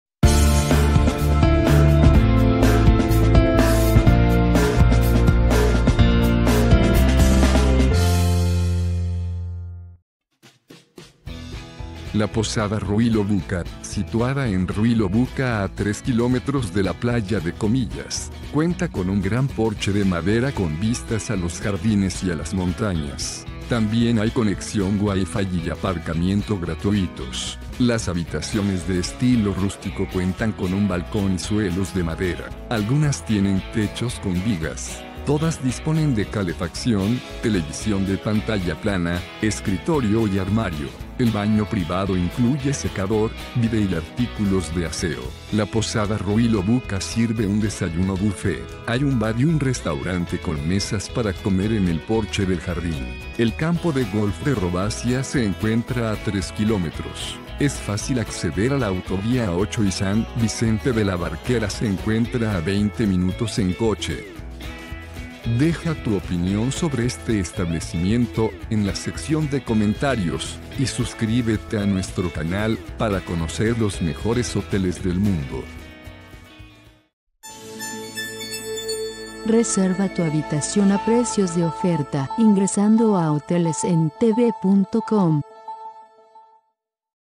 Posada Ruilobuca en Ruiloba
Posada Ruilobuca es un Hotel ubicado en Pomar, 17, Barrio Ruilobuca. Este encantador Hotel cuenta con una ubicación privilegiada en la ciudad de Ruiloba. Este alojamiento alberga 9 habitaciones con secador de pelo, balcón y escritorio.